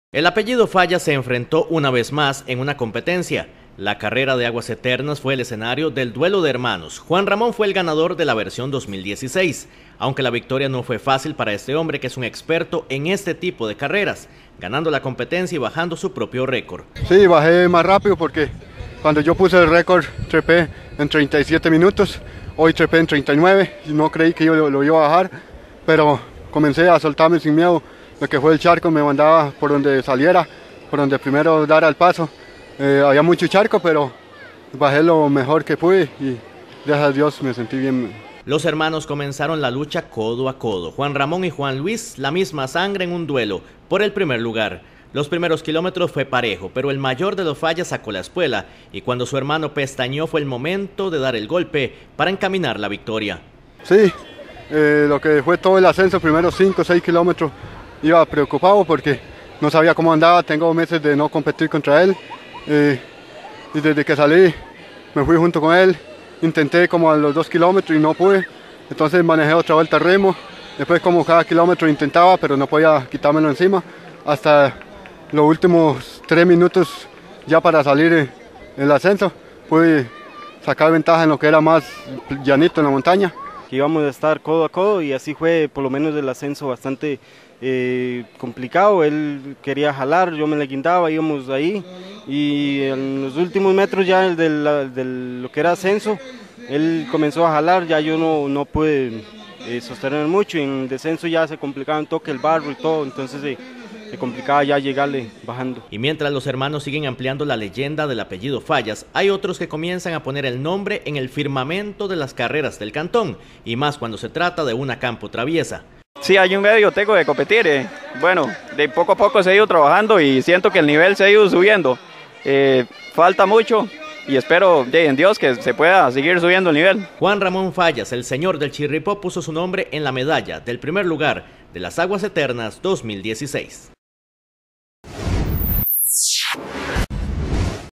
Hermanos Fallas lucharon en las aguas eternas
El apellido Fallas se enfrentó una vez más en una competencia, la carrera de aguas eternas fue el escenario del duelo de hermanos.